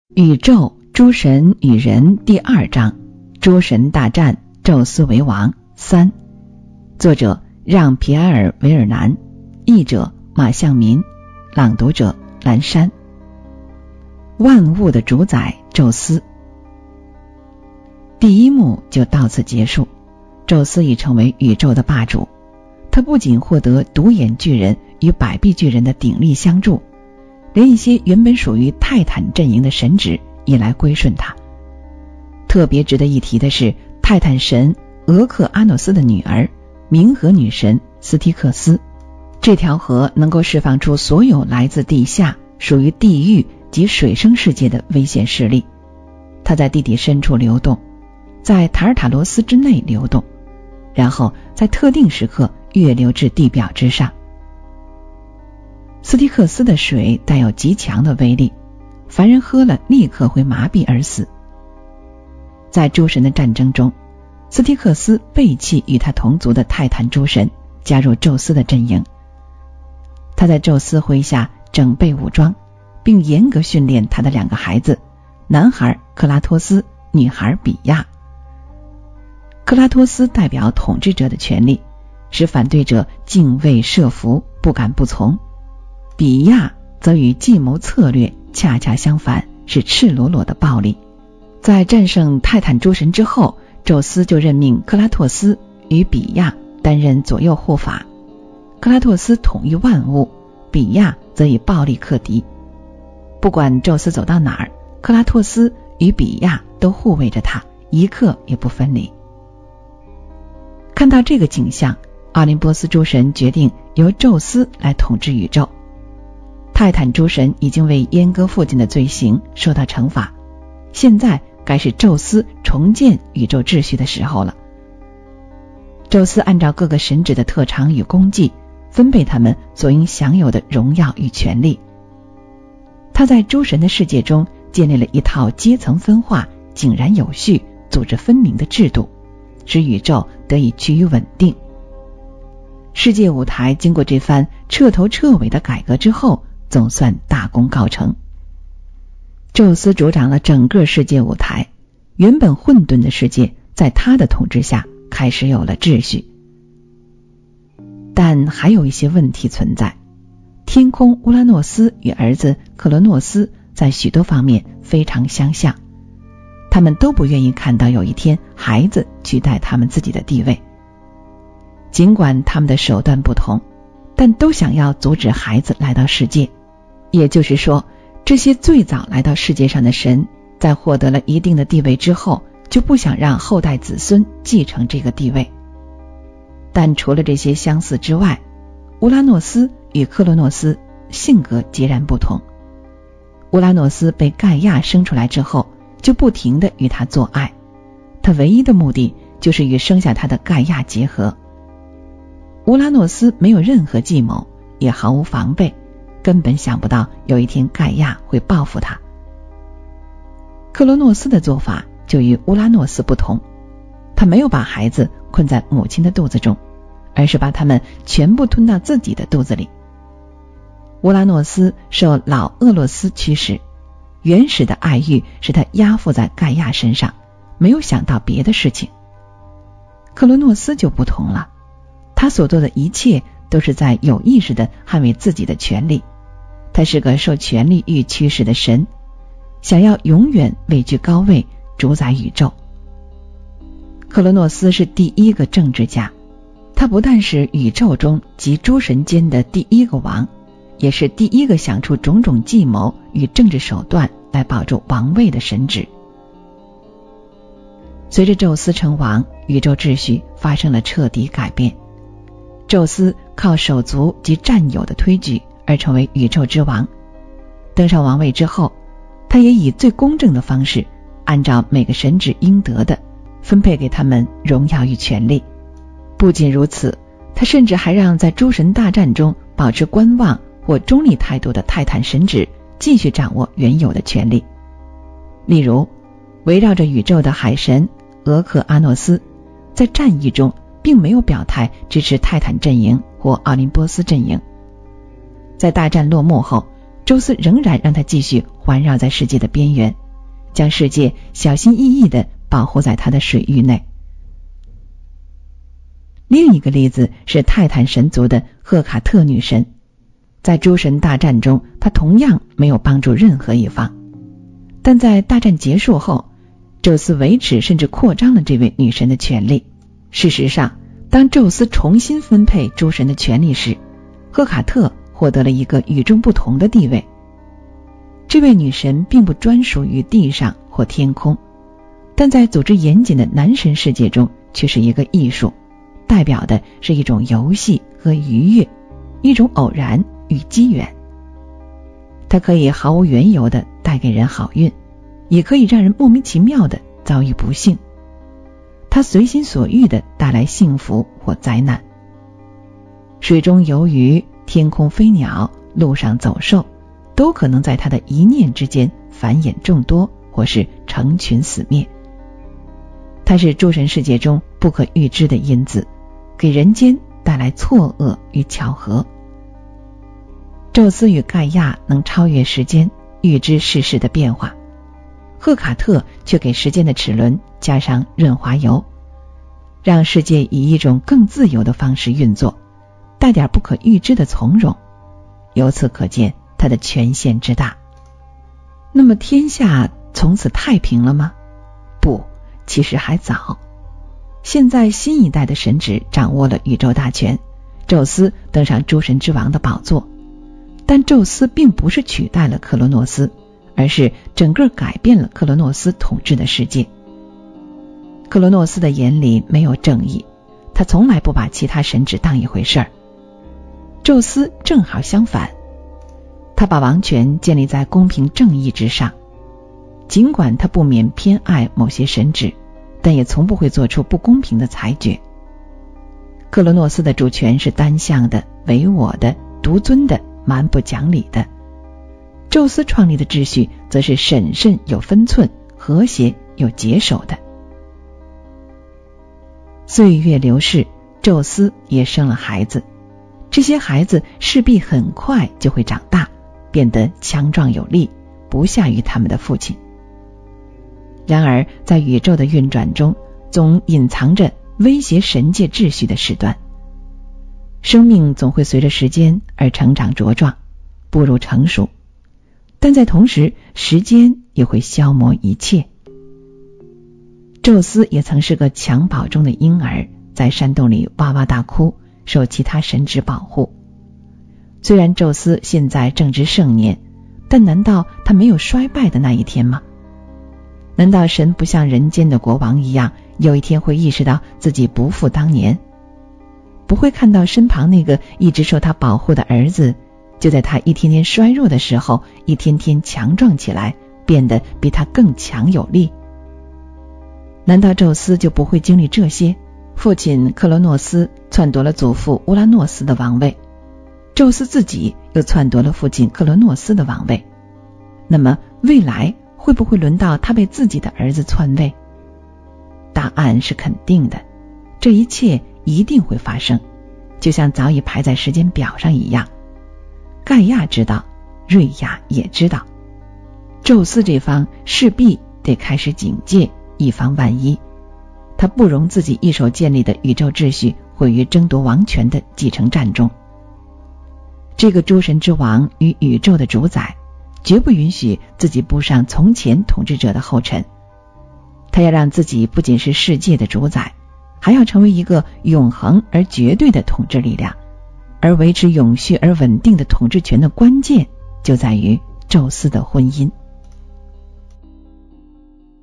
08 万物的主宰：宙斯 | 《希腊神话：宇宙、诸神与人》| 韦尔南 | 阑珊
一次偶然的机会，古希腊研究大师韦尔南用说书人的口吻给小孙子讲起了希腊神话，孩子眼中的快乐让这位大师埋首书斋所作的研究考据焕发出新的光芒。于是他开始重新整合信息，尝试以深入浅出的语言引导读者，以打通希腊神话对人与宇宙、人对他人、人对自身的认知之间的关联。